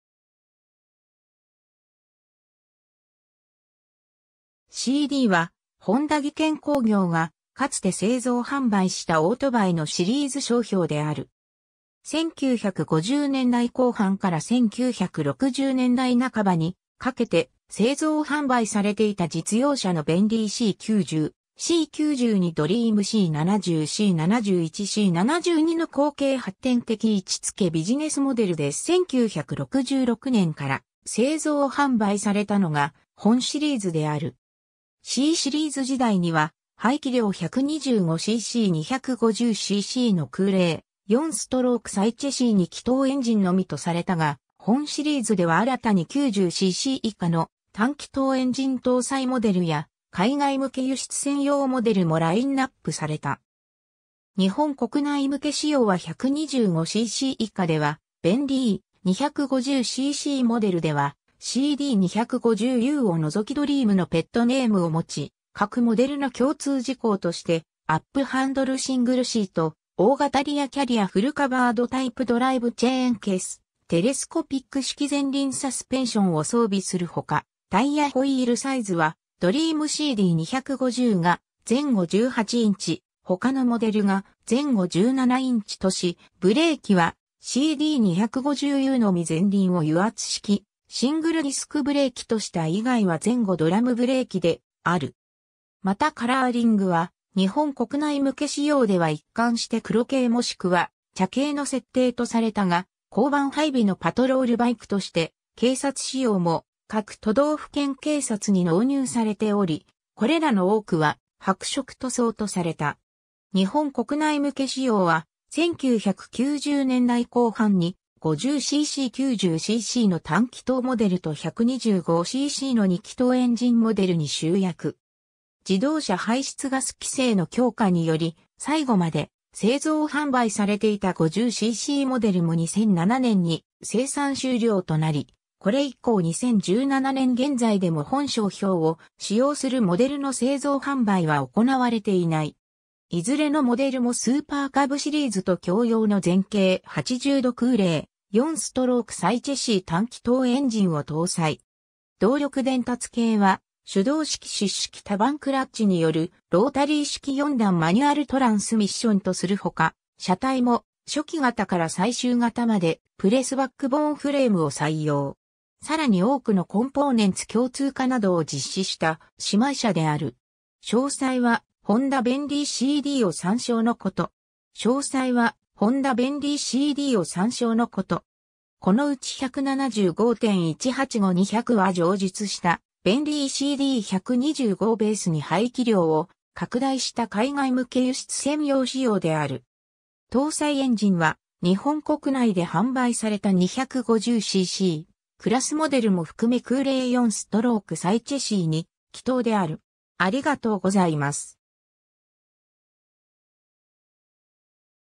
ホンダ・CD
CD(シーディー)は、本田技研工業がかつて製造販売したオートバイのシリーズ商標である。
 1950年代後半から1960年代半ばにかけて製造販売されていた実用車のベンリイC90・C92・ドリームC70・C71・C72の後継発展的位置付ビジネスモデルで1966年から製造販売されたのが本シリーズである。
Cシリーズ時代には排気量125cc（小型自動二輪車）・250cc（普通自動二輪車）の空冷4ストロークSOHC2気筒エンジンのみとされたが、本シリーズでは新たに90cc以下の単気筒エンジン搭載モデルや海外向け輸出専用モデルもラインナップされた。
 日本国内向け仕様は125cc以下ではベンリイ、250ccモデルではCD250Uを除きドリームのペットネームを持ち、各モデルの共通事項としてアップハンドル・シングルシート・大型リヤキャリア・フルカバードタイプドライブチェーンケース・テレスコピック式前輪サスペンションを装備するほか、タイヤ・ホイールサイズはドリームCD250が前後18インチ、他のモデルが前後17インチとし、ブレーキはCD250Uのみ前輪を油圧式シングルディスクブレーキとした以外は前後ドラムブレーキである。
またカラーリングは日本国内向け仕様では一貫して黒系もしくは茶系の設定とされたが、交番配備のパトロールバイクとして警察仕様も各都道府県警察に納入されており、これらの多くは白色塗装とされた。
 日本国内向け仕様は、1990年代後半に50cc（原動機付自転車）・90ccの単気筒モデルと125ccの2気筒エンジンモデルに集約。
自動車排出ガス規制の強化により最後まで製造販売されていた50ccモデルも2007年に生産終了となり、これ以降2017年現在でも本商標を使用するモデルの製造販売は行われていない。
 いずれのモデルもスーパーカブシリーズと共用の前傾80°空冷4ストロークSOHC単気筒エンジンを搭載。
動力伝達系は手動式湿式多板クラッチによるロータリー式4段マニュアルトランスミッションとするほか、車体も初期型から最終型までプレスバックボーンフレームを採用。
さらに多くのコンポーネンツ共通化などを実施した姉妹車である。
 詳細はホンダ・ベンリィ#CDを参照のこと。
 このうち175・185・200は上述したベンリイCD125（T）をベースに排気量を拡大した海外向け輸出専用仕様である。
搭載エンジンは日本国内で販売された250㏄クラスモデルも含め空冷4ストロークSOHC2気筒である。